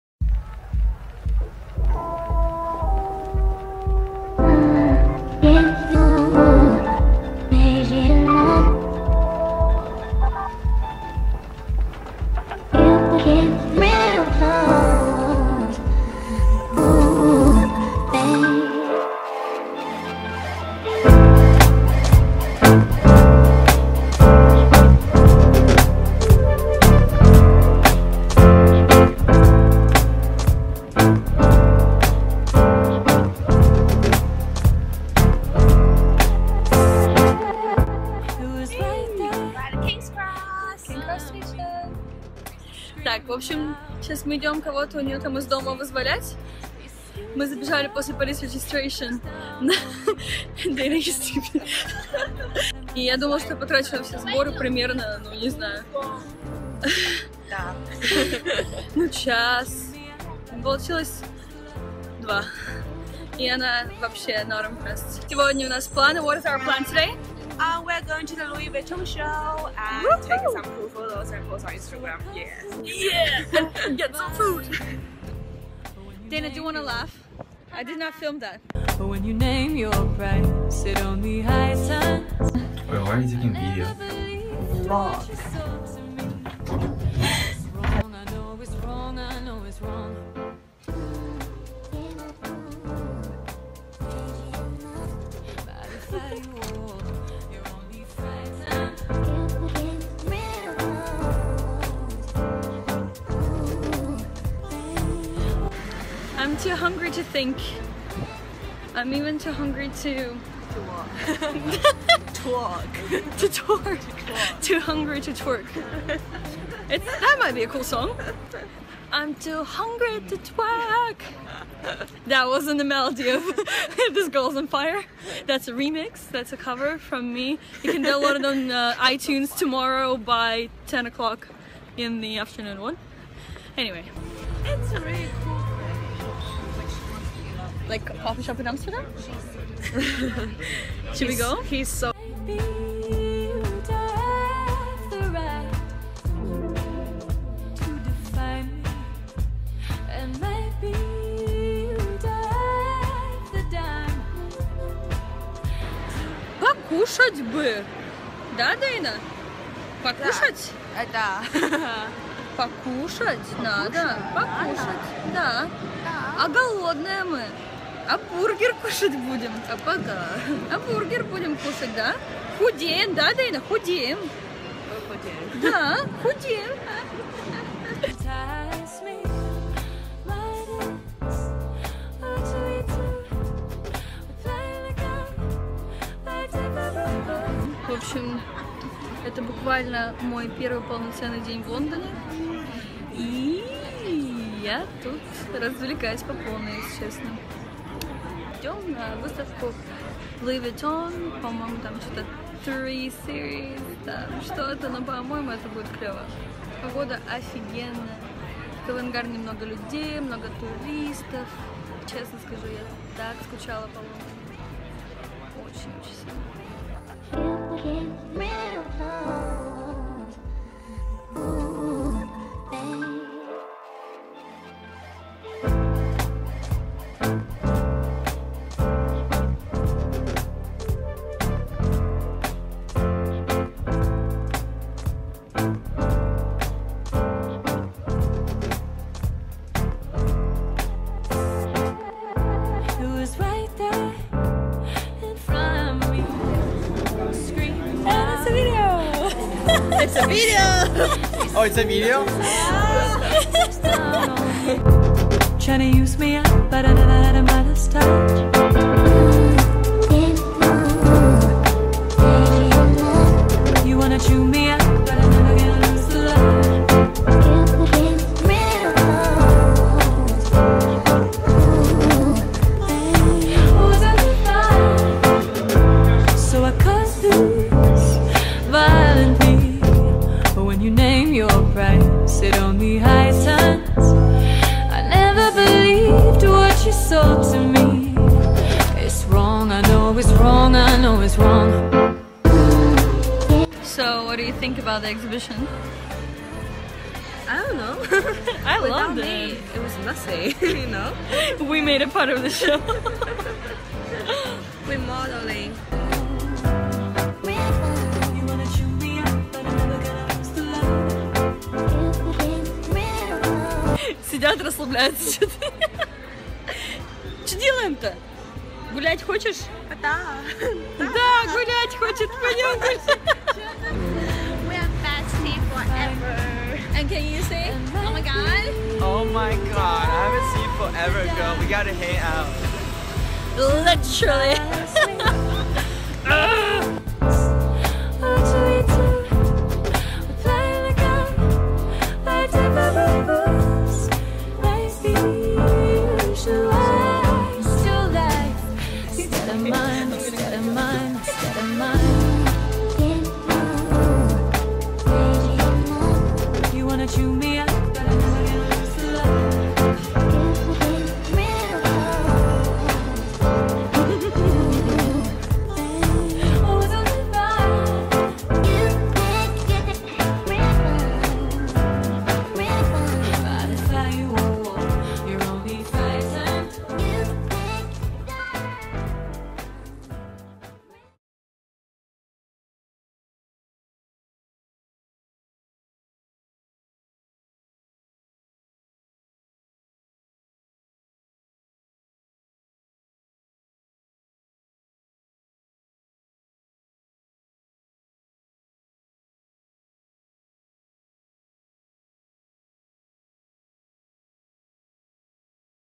//VLOG 1// Back to LONDON, TOO HUNGRY TO TWERK и Louis Vuitton Series 3 Exhibition
Это мой самый первый ВЛОГ! Здесь я снимаю что со мной и моими друзьями происходит в повседневной жизни в Лондоне.

This is my first vlog EVER! In this vlog I'm showing whats happening to me and my friend in our daily life in London.